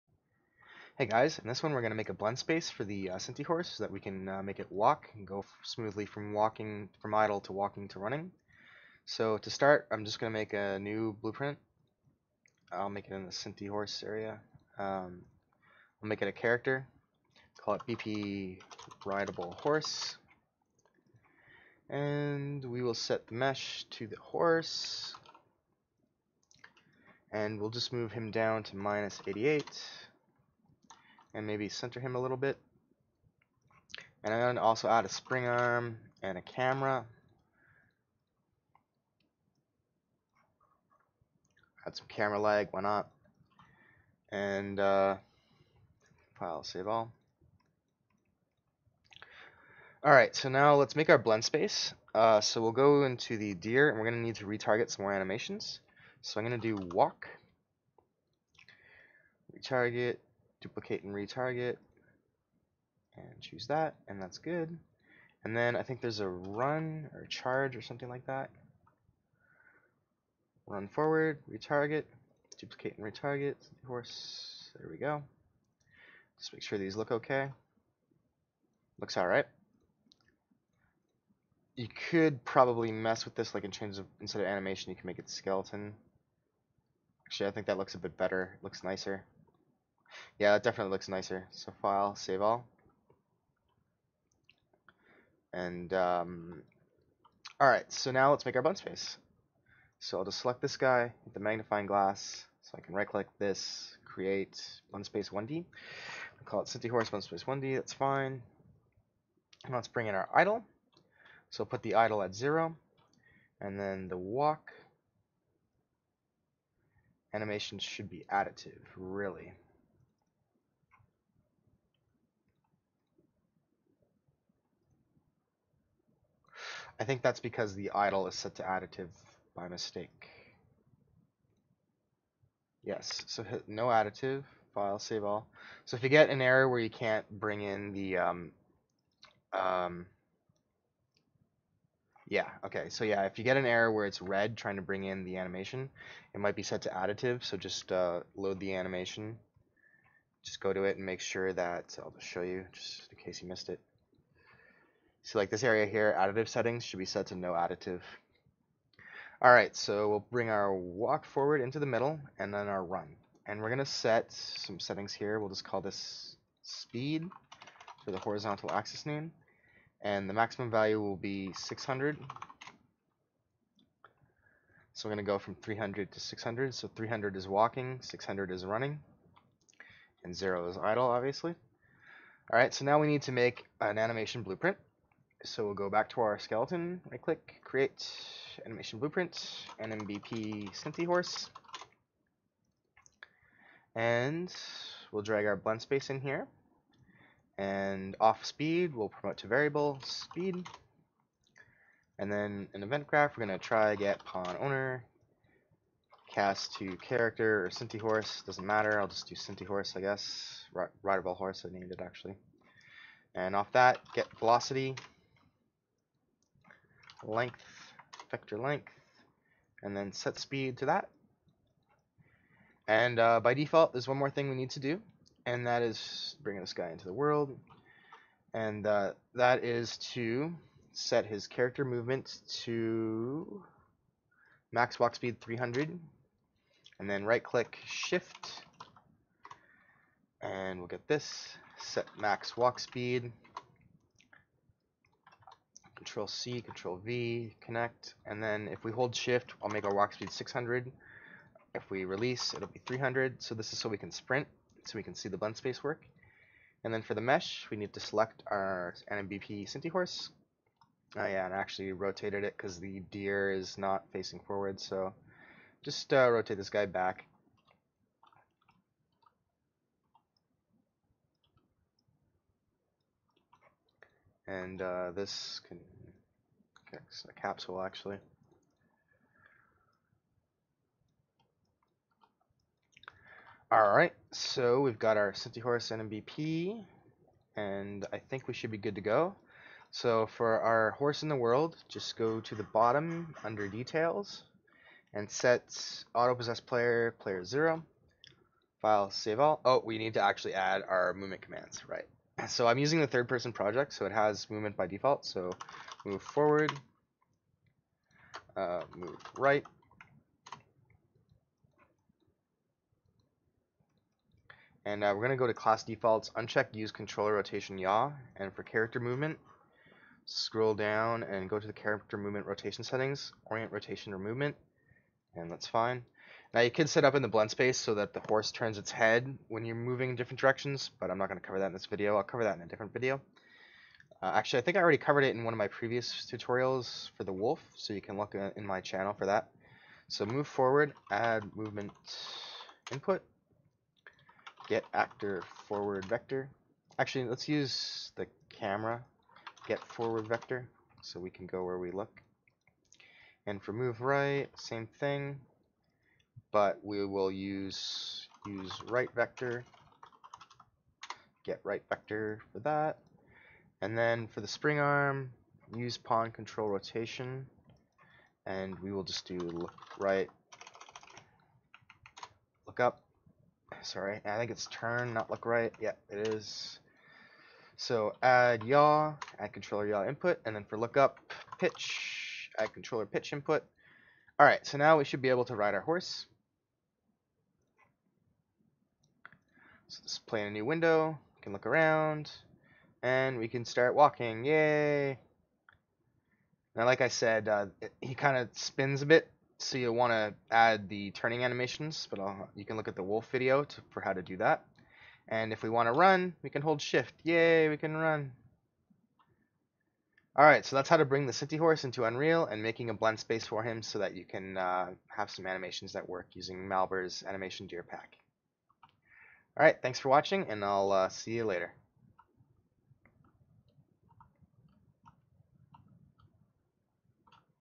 Bringing Synty's Polygon Horse form Unity to Unreal (part 2)
In this part we set up a rideable horse with animation blendspaces